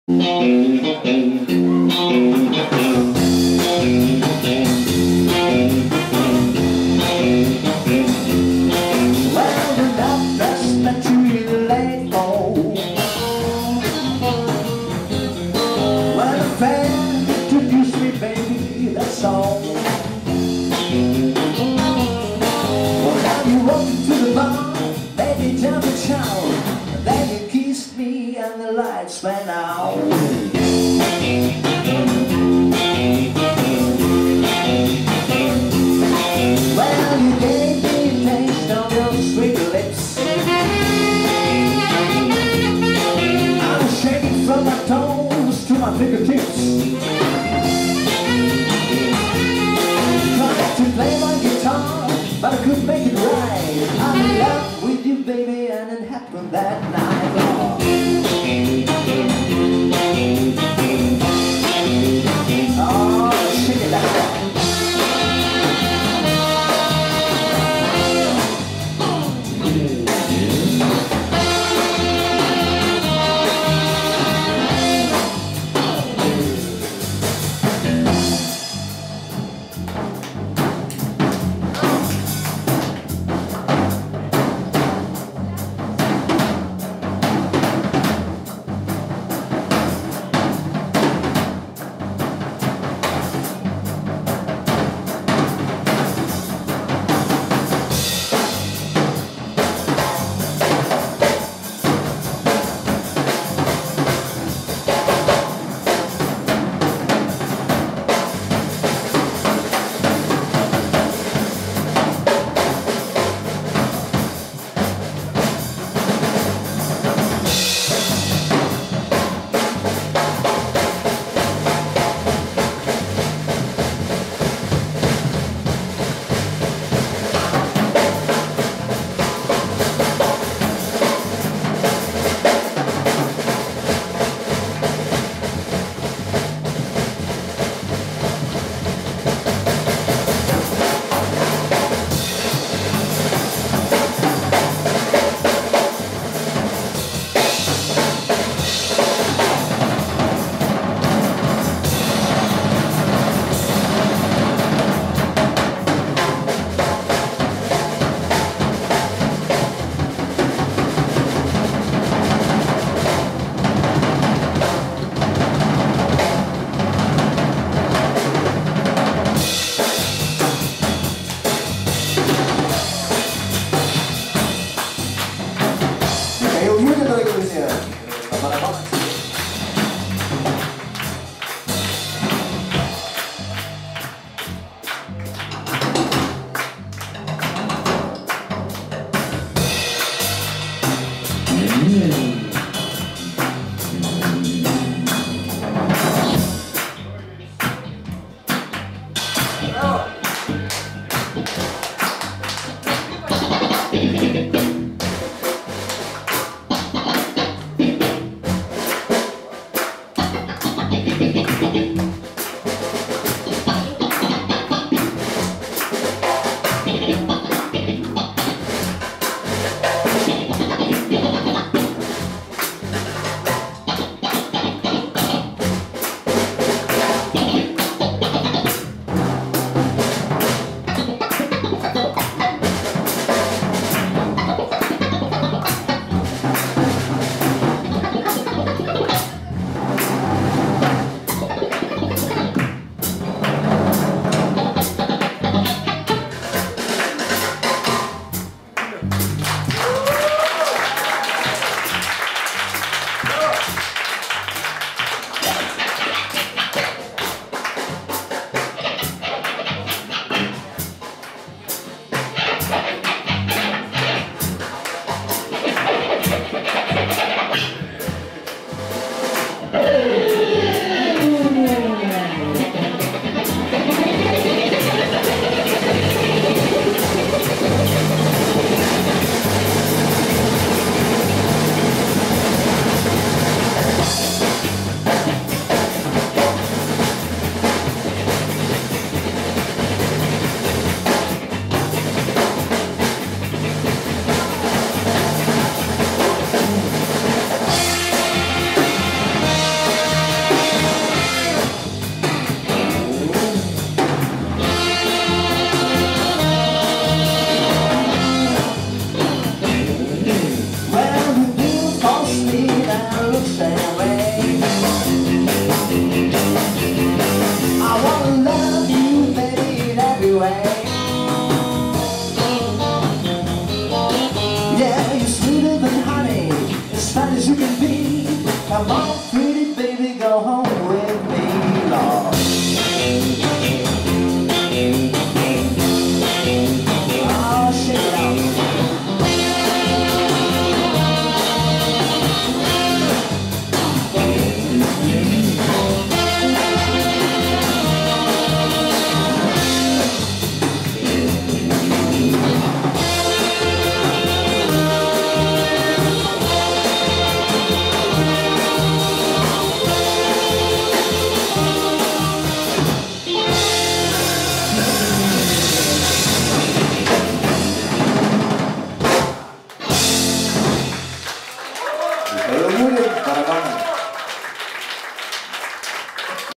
Вадим Иващенко & The Boneshakers with Михаил Шишкин
Live at the Roadhouse Blues Club, July 4, 2015